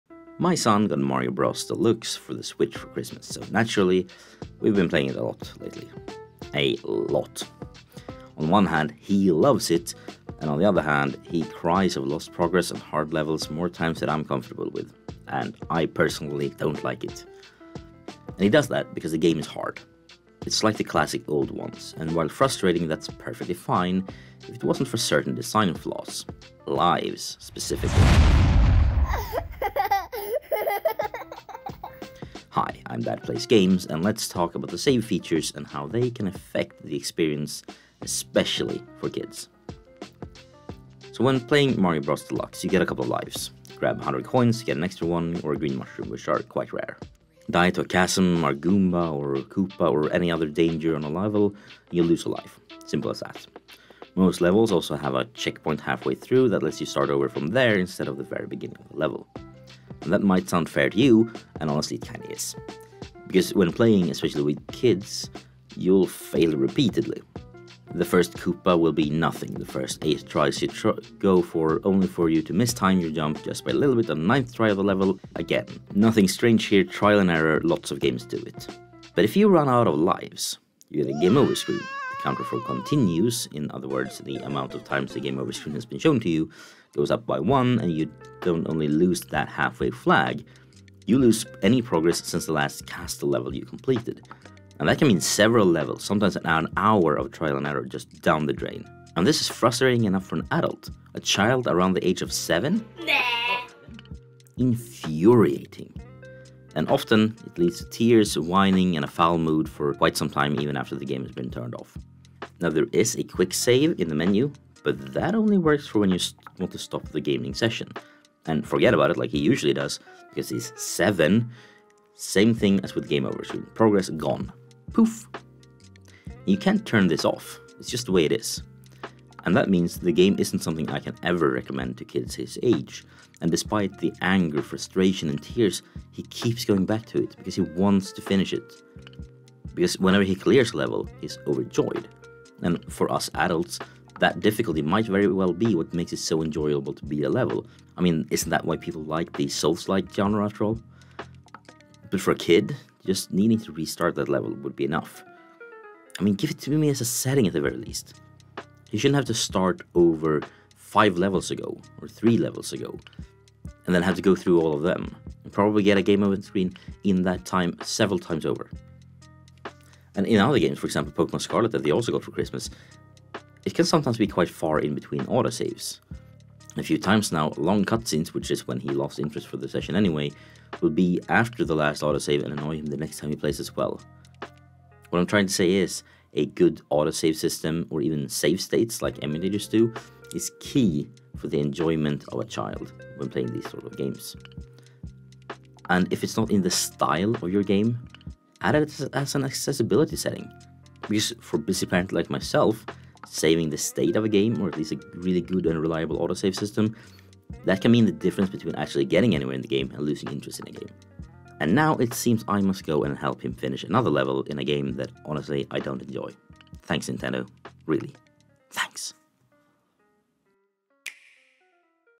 Save points can ruin a game
A game can be great, except for a bad save system and it can ruin the whole experience. Especially for a kid. My son recently got a new Mario game and... It's infuriating!